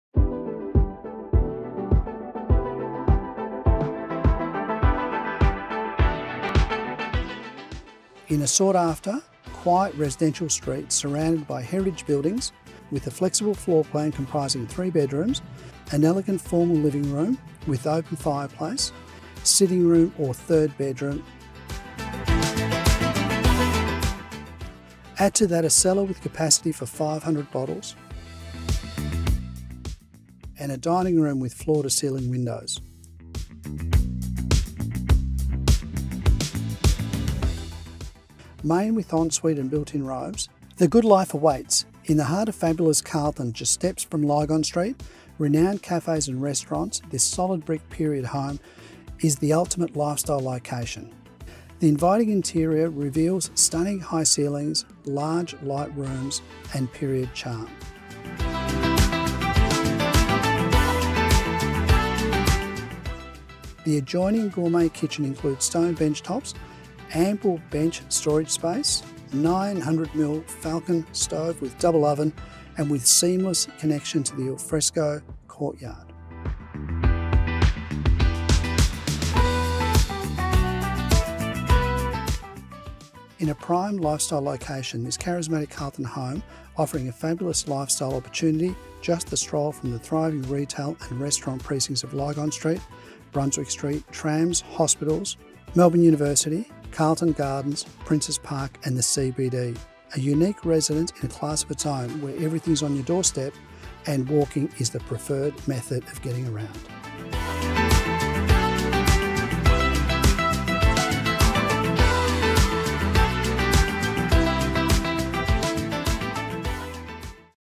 384 Drummond Street, Carlton For Sale by Tom Alexiadis
or call Tom Alexiadis on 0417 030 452.

In the heart of fabulous Carlton just steps from Lygon Streets renowned cafes and restaurants this solid brick period home is in the ultimate lifestyle location. In a sought after, quiet residential street, surrounded by heritage buildings, this period home c1857 was formerly a haberdashery shop and has now been converted to a stylish, three-bedroom, architect-designed residence with rear laneway access. The inviting interior reveals stunningly high ceilings, large light rooms and period charm.